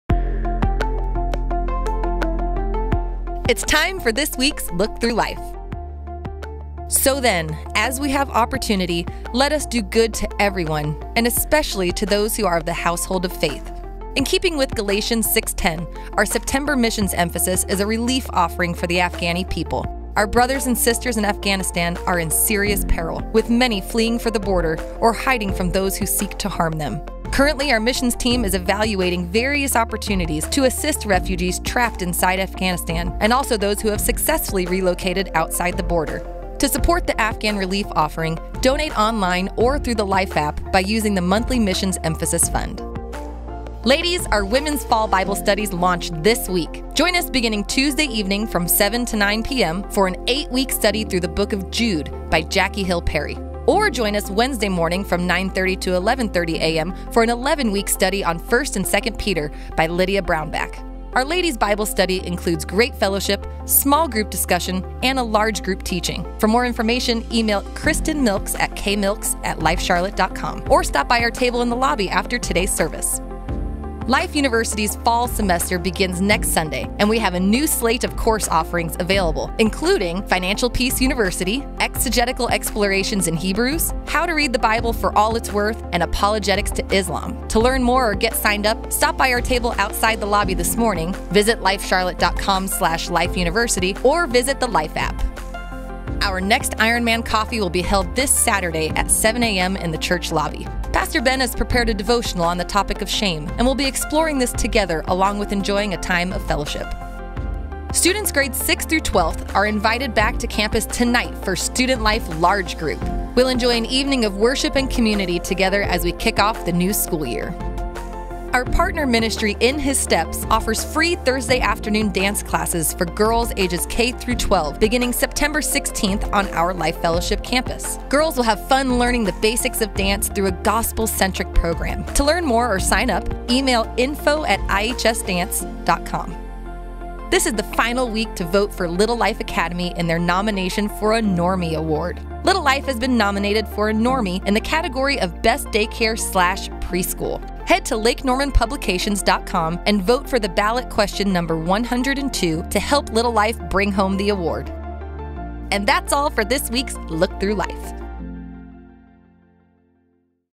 New Missions Emphasis!
Learn how you can make a difference in this month's missions emphasis and get the details on this week's ministry opportunities.